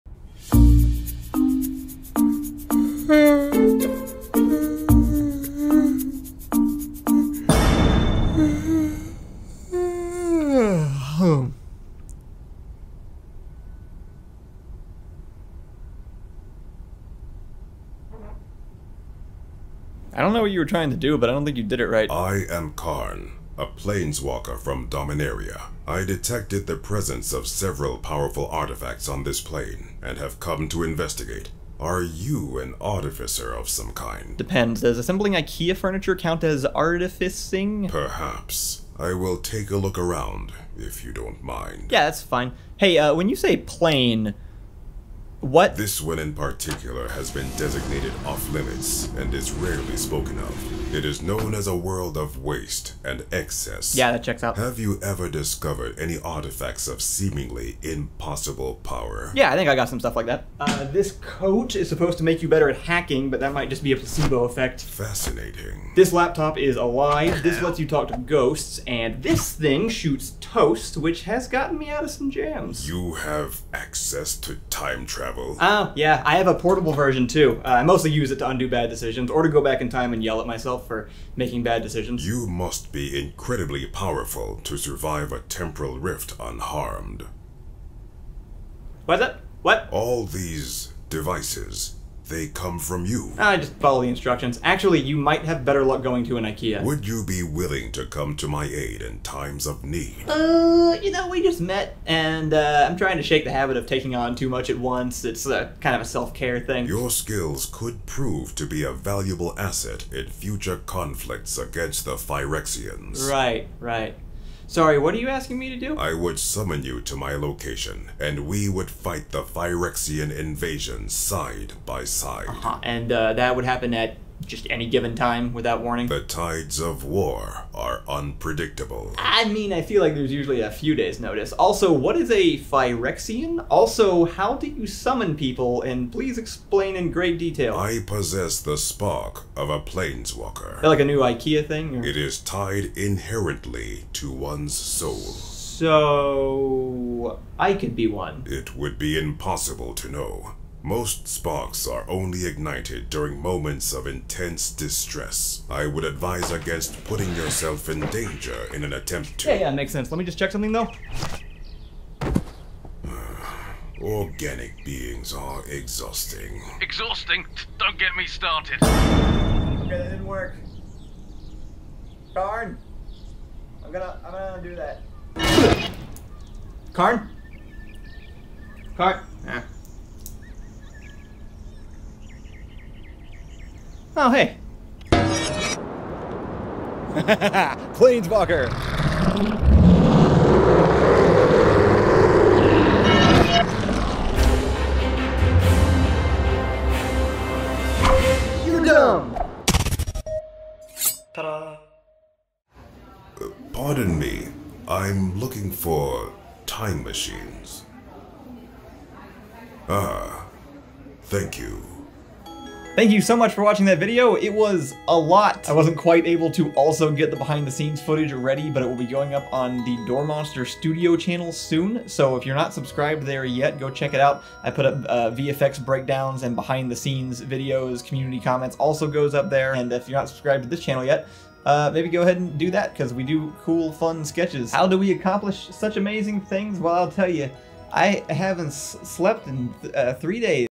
Self-Fulfilling Idiocy 3 (2): Planeswalkin'
Kyle meets an interdimensional traveler.

Starring Kyle and Blender
with special guest Squire
Costume inspired by Dom Atlas, @AtlasCrafts